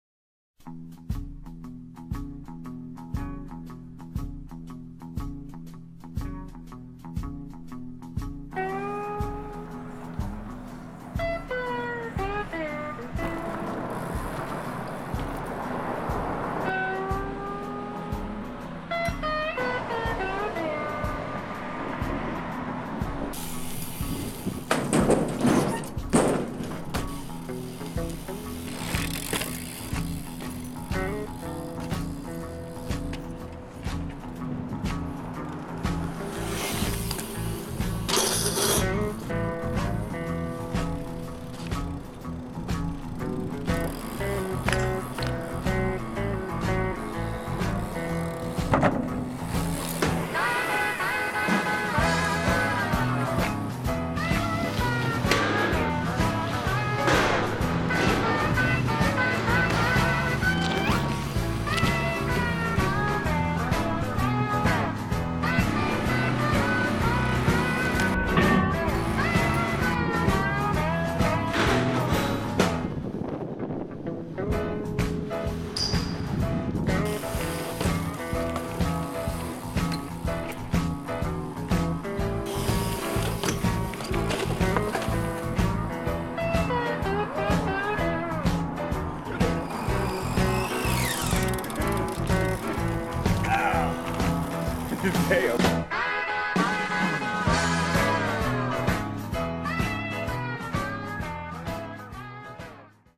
FBM-Philthy Session!
A Sunday session with Kevin Mitchell and Phil Jones hitting local spots in Charlotte, NC. Enjoy!
9 / 13 / 15 Session / A Philthy Feature